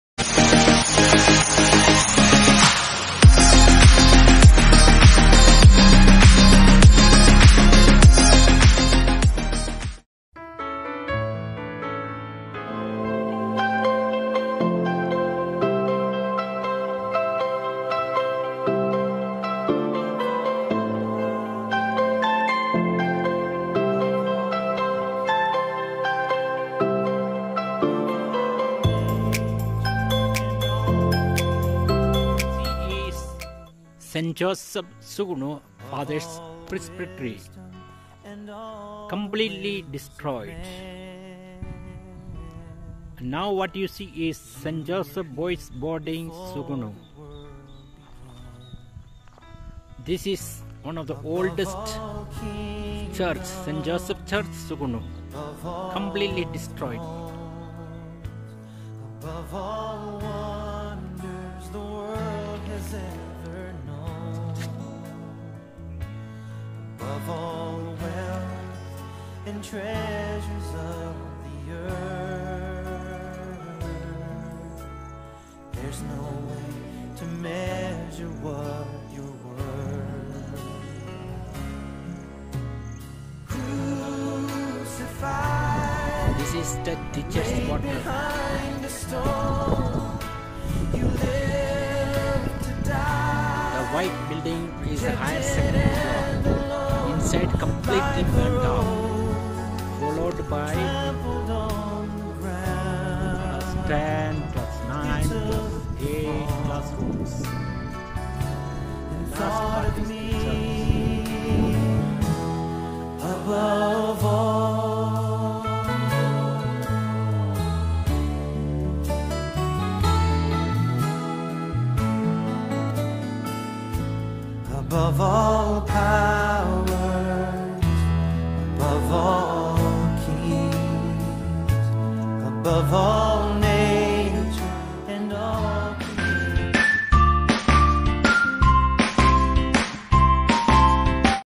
The Second Oldest Catholic School & Parish in Manipur is on fire 🔥# .
The Second Oldest Catholic School & Parish in Manipur is on fire again for the second time 💔🥺which was established in 1969 which is located at Sugnu.
Father, forgive them, for they do not know what they are doing'” (Luke 23:34).
St. Joseph PRAY FOR US 🙏🏻❤️

Vocal:Rev. Fr. Roy.
EDITED VIDEO.